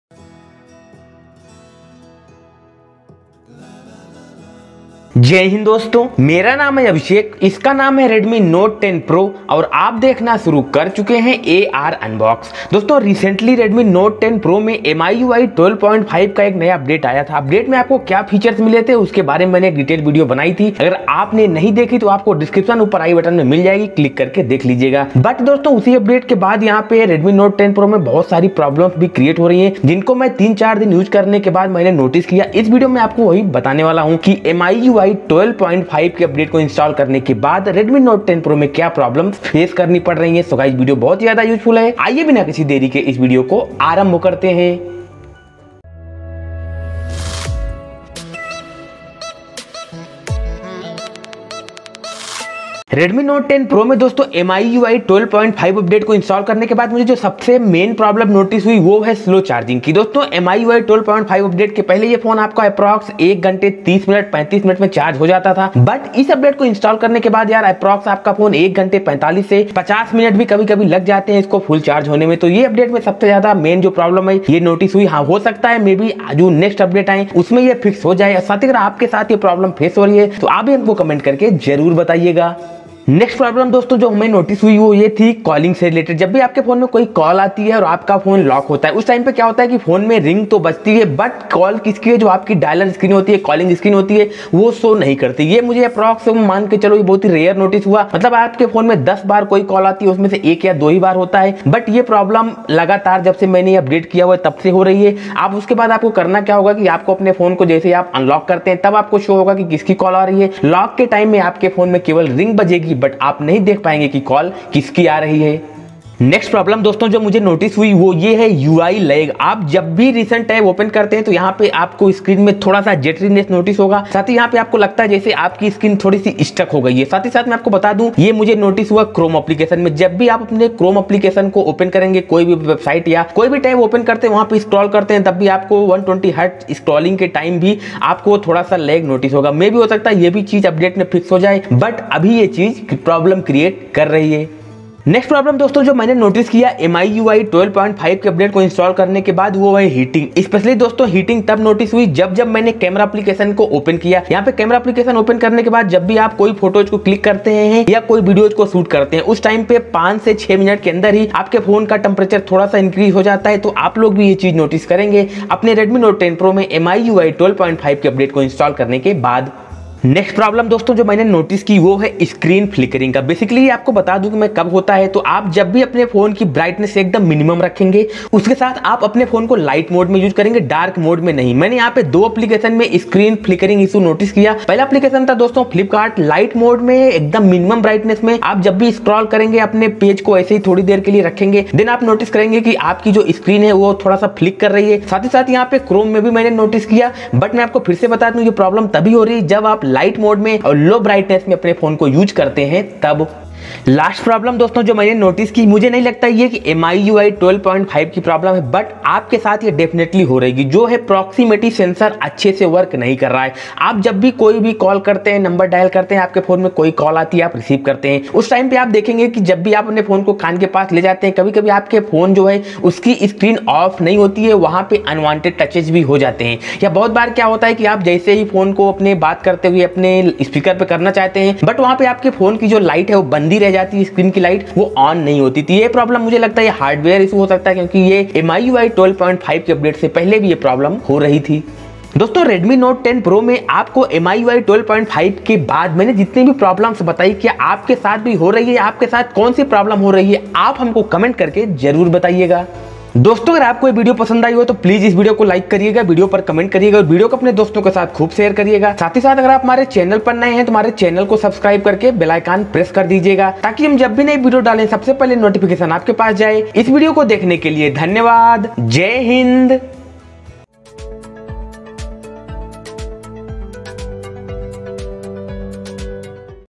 Redmi Note 10 Pro MIUI 12.5 Big Problems | Redmi Note 10 Pro MIUI 12.5 Bugs,Screen Flickering
Hi Guys,
Aj es video me hum discuss krene wale hai Redmi note 10 pro me MIUI 12.5 Update ke bad top 5+ problems jo redmi note 10 pro me notice ho rhi h. So Guy's vidoeo bhut useful hai end tak jaroor dekhna.

Gear Use In Video -
Smartphone Mi A3
HP Laptop
Boya M1 Mic
Gorilla Pod
Adofys Tripod

Chapters
00:00 Intro
00:53 Slow Charging
01:23 Calling Screen Not Showing
01:56 UI Lag
02:25 Heating
02:50 Screen Flickering
03:24 Proximity Sensor Issue
04:13 Outro